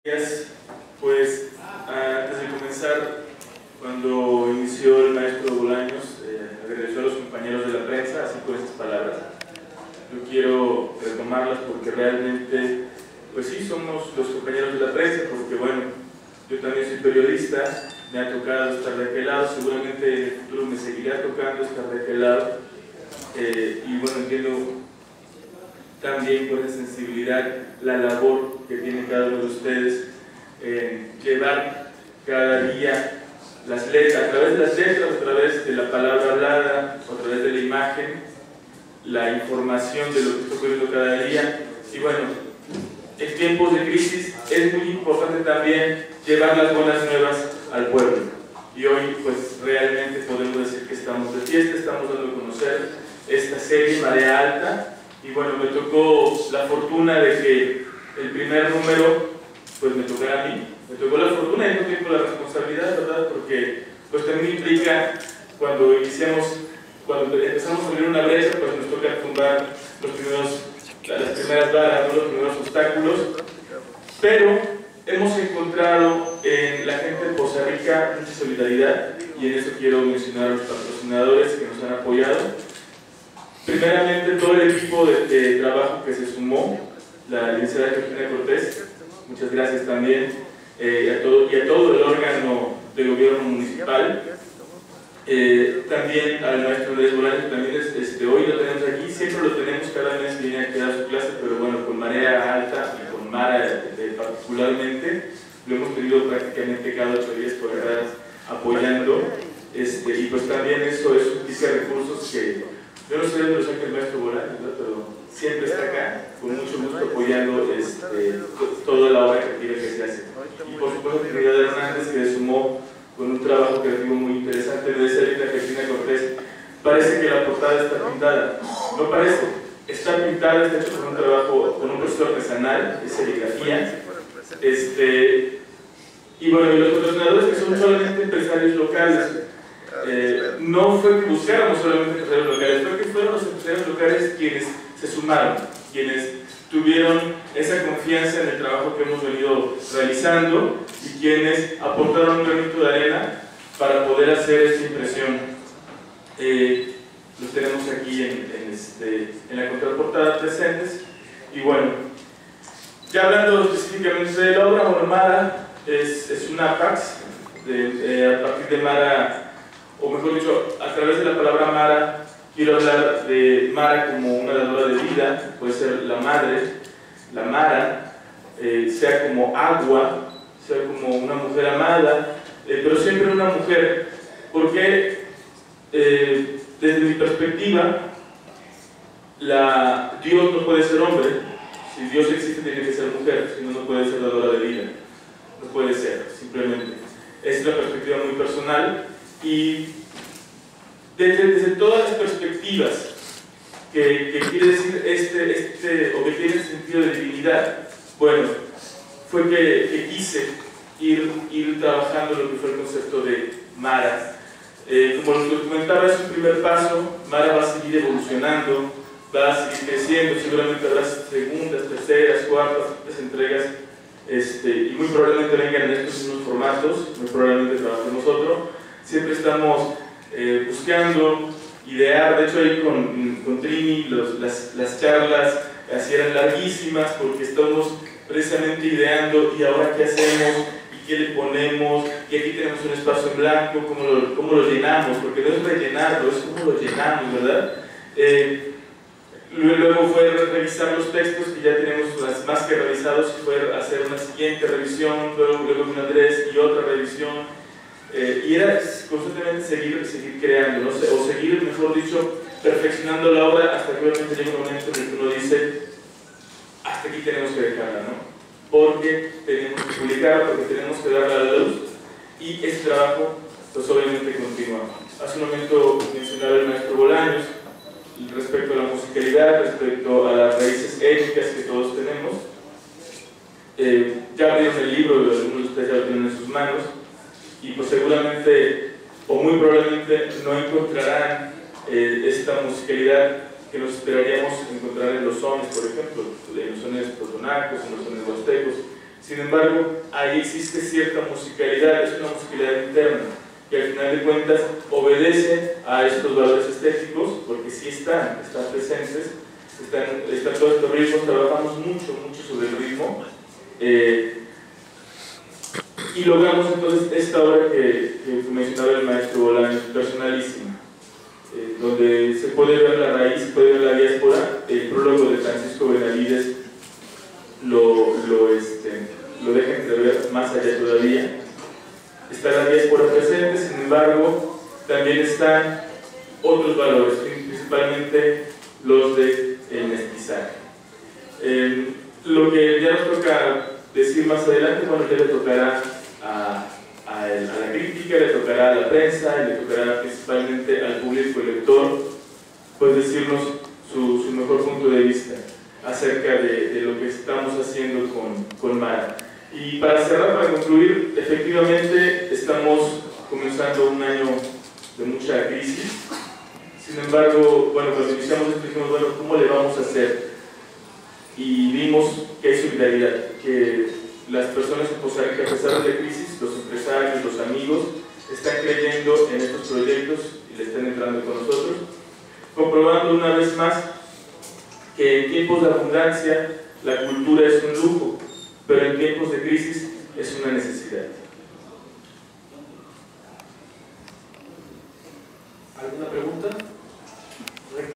Ramon Rosas Caro escritor presentación de sus textos Mara
Presentación del Cuadernillo Mara
obra poética del escritor Ramón Rosas Caro